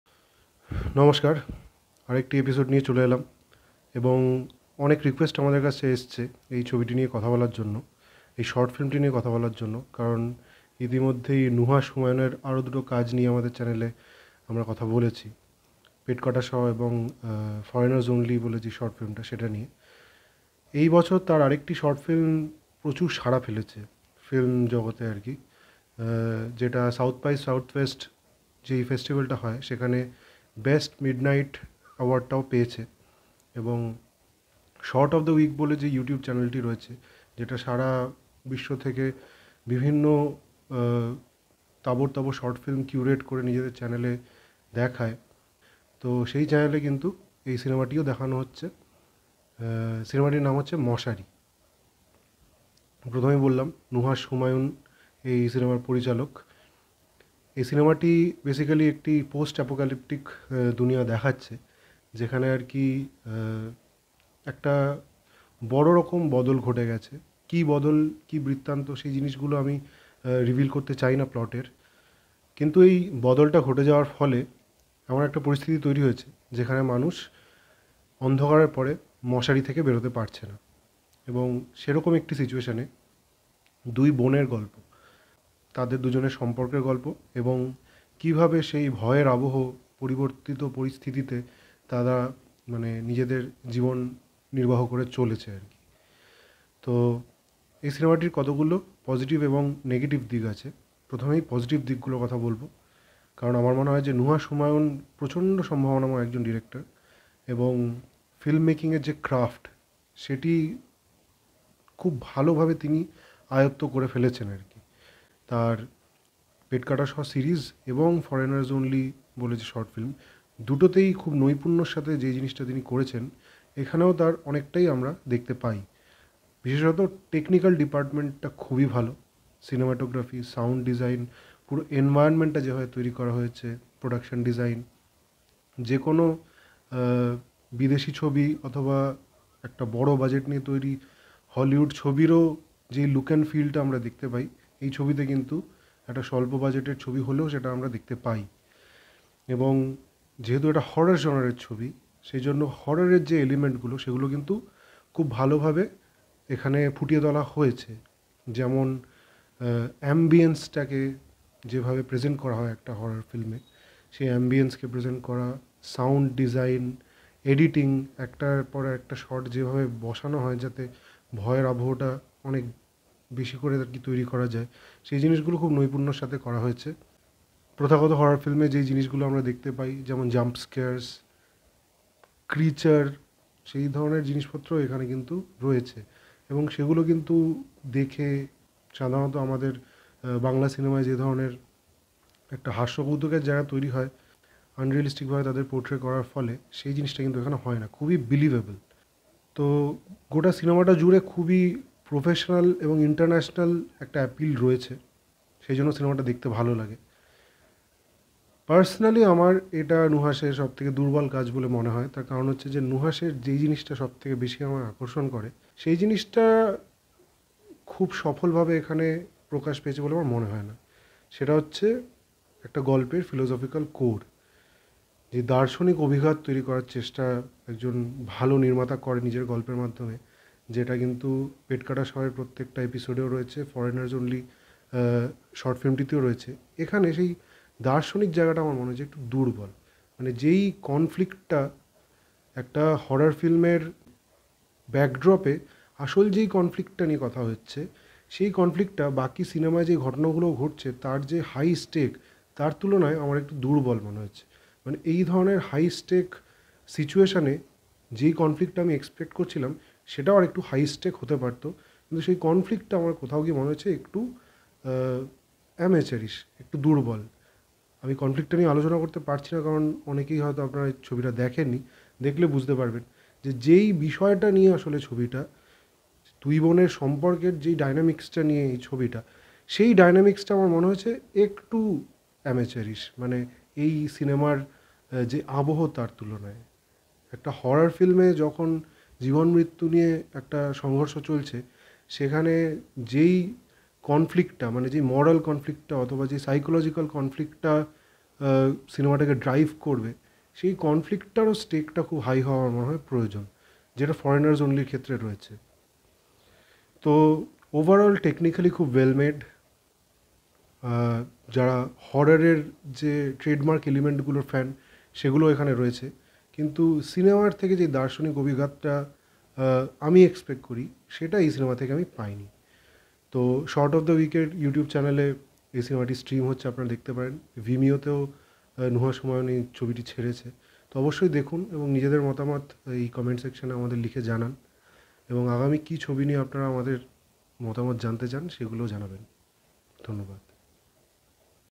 Moshaari AWARD WINNING Short Film Review | Nuhash Humayun
In this video we review Nuhash Humayun's short film Moshaari